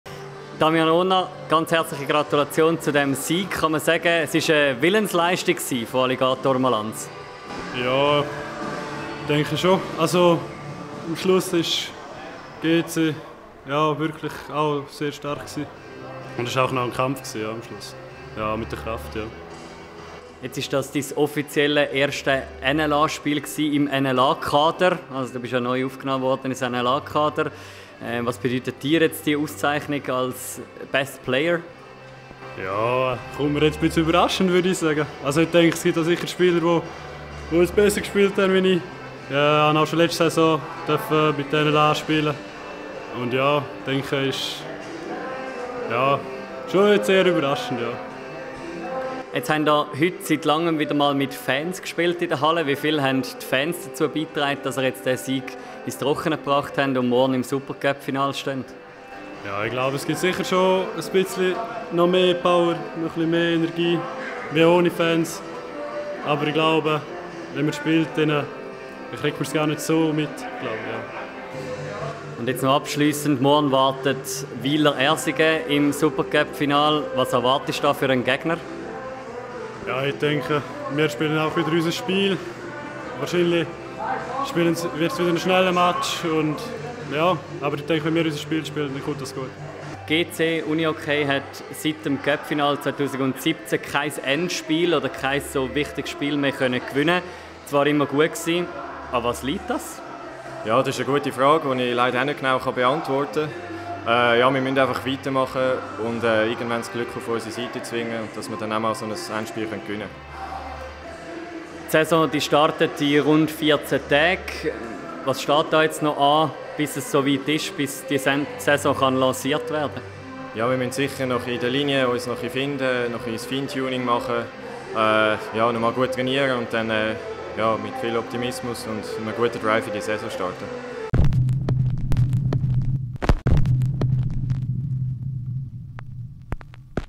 Interviews Halbfinal GC Unihockey vs. UHC Alligator Malans – 2:3
Best Player Damian Rohner und Florian Wenk im Interview